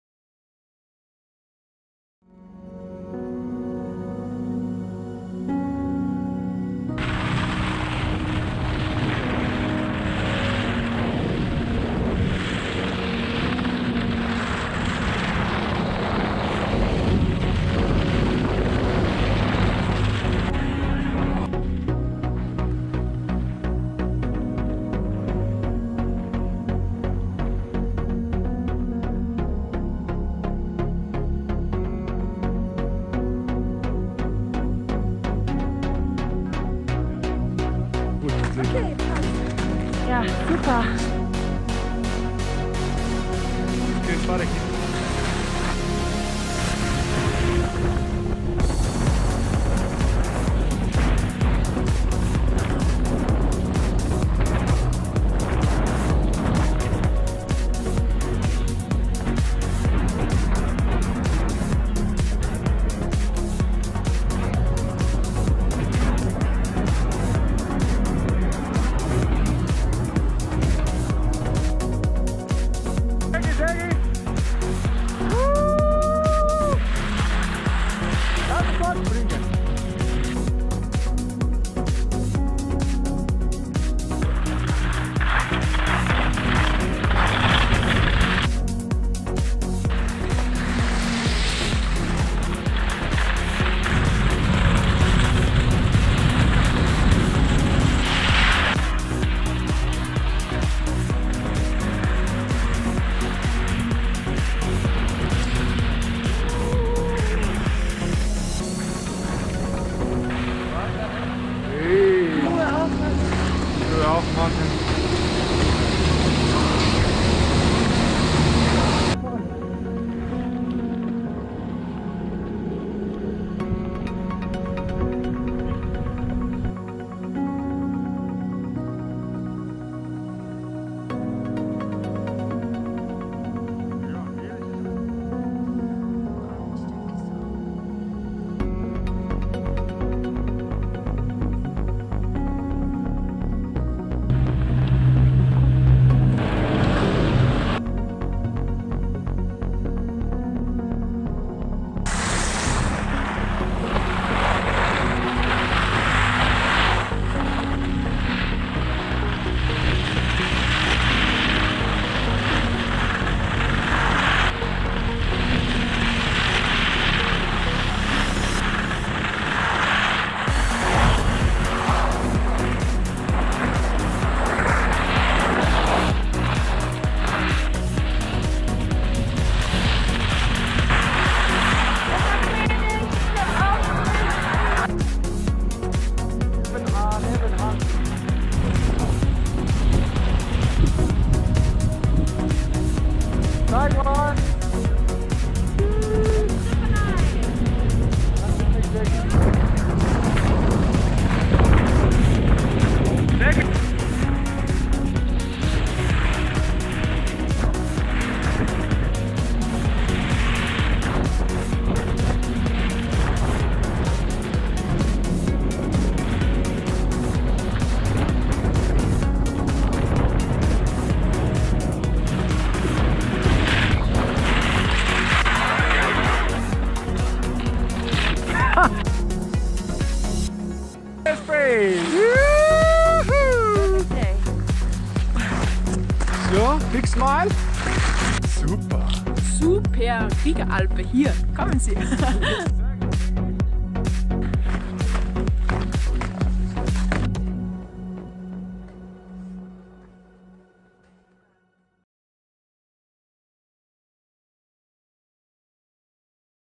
Go ski be happy, short and long ski turns
Thanks Zeggy and Iris for this last ski day in Lech am Arlberg ski resort in Austria.So missing my job and friends form winter time I made this video.In our ski shop you can find good ski stuff and more.
by buying something you supporting our channel so we can make even more videos.

Iris has Rossi master gs 180 ski.
Zeggy has Head ispeed pro 185cm ski.
Miromiro on Fischer gs master 183cm ski.